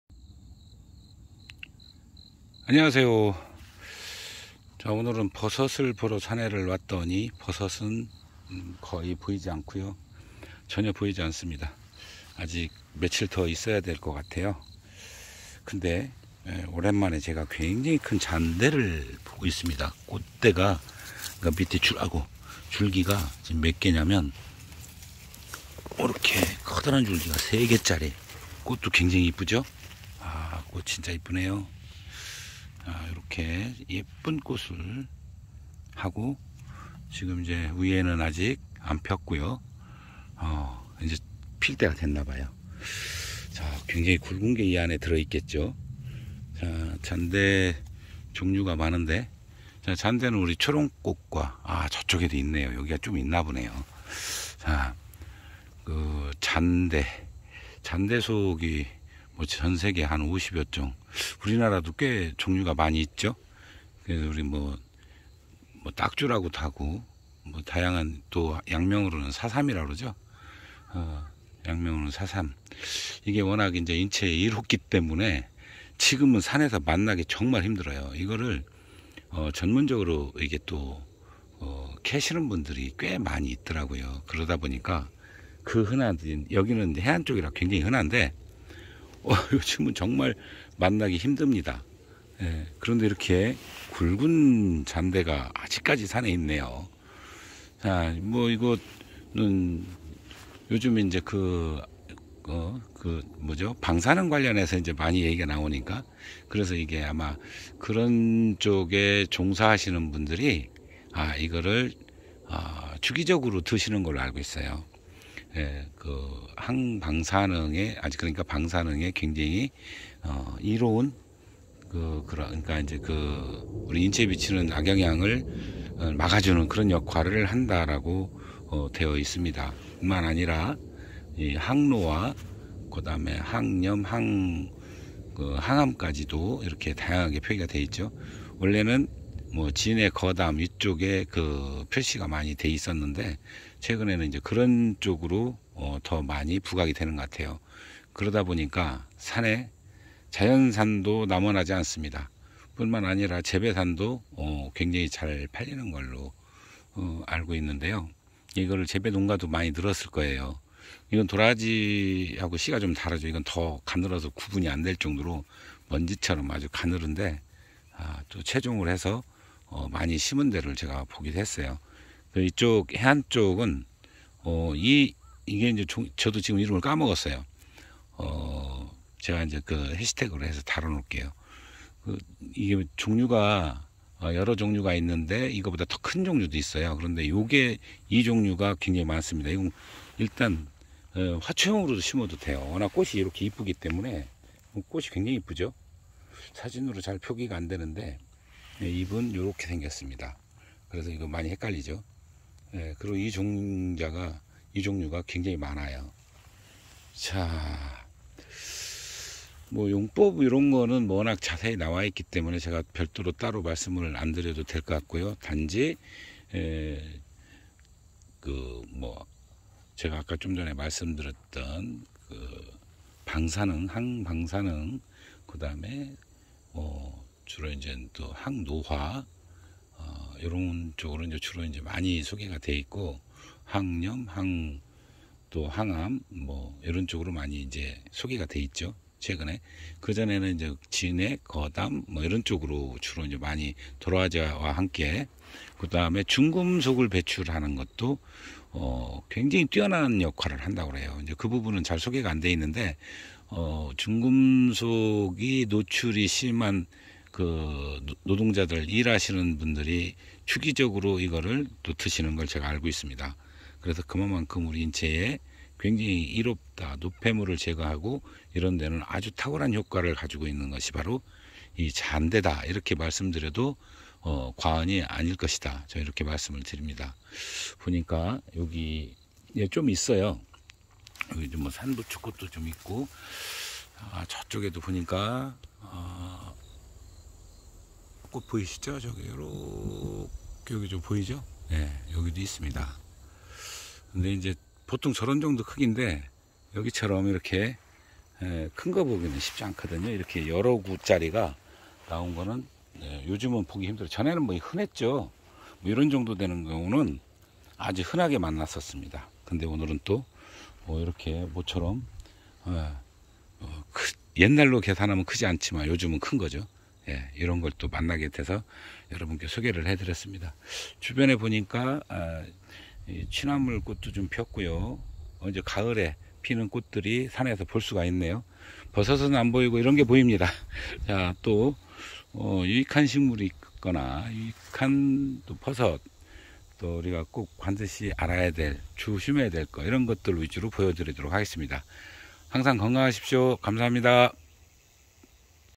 852편, 해독작용이 뛰어난 요즘 보기 힘든 대물 잔대가~
산행중 굵은 싹대가 여러대가 나온 대물급 야생잔대가 보여 효능과 함께 소개하여 드립니다.